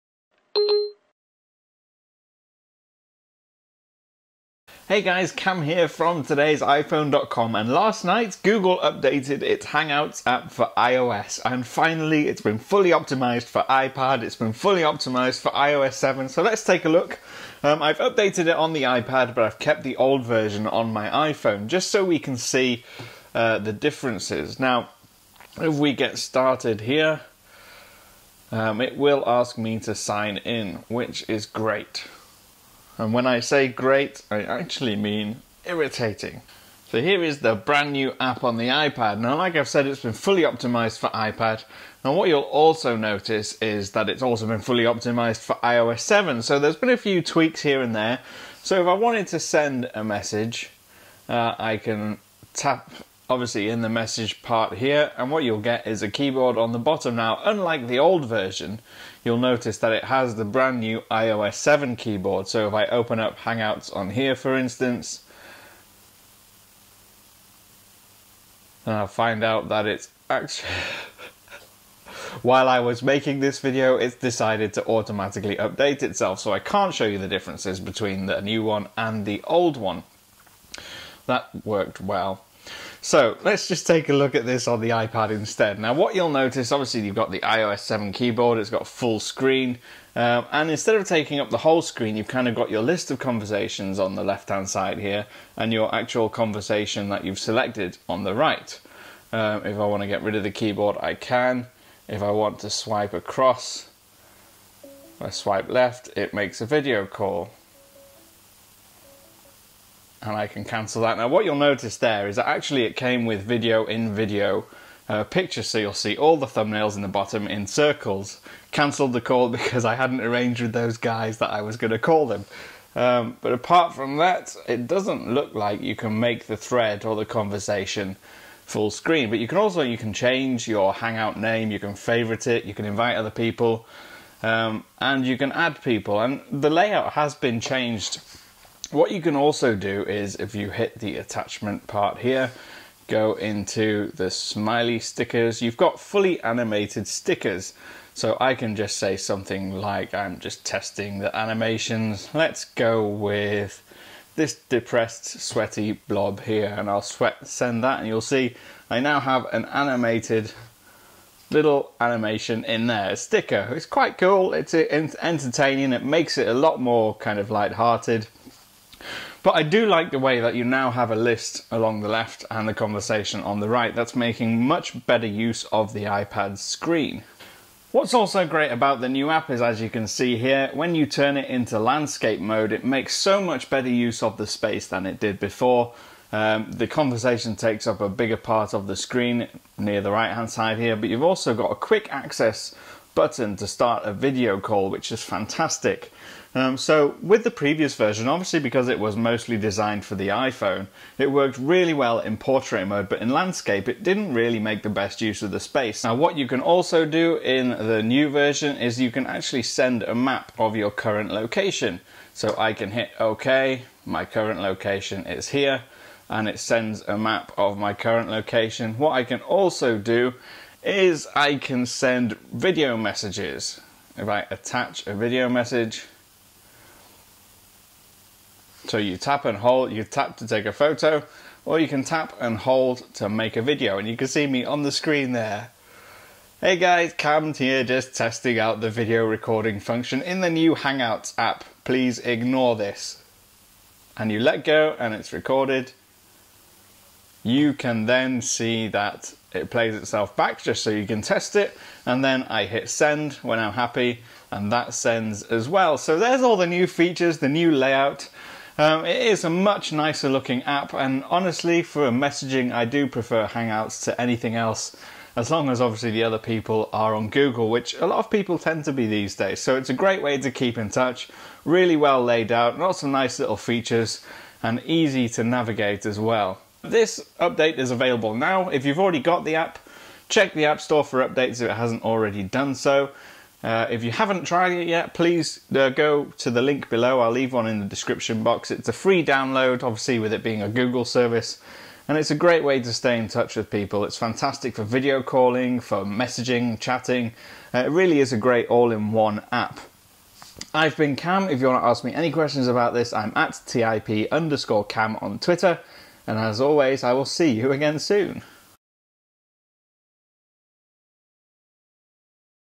Google Hangouts 2.0 for iPad and iPhone - Overview
Cam takes a look at the brand new Hangouts for iPad and iPhone. It's available for free on the App Store now. New features include:

-- Fully-optimized for the iPad, including picture-in-picture video calling
-- Animated stickers that help you share exactly how you're feeling
-- Record and send video messages up to 10 seconds long
-- Send a map of your current location
-- Optimized for iOS 7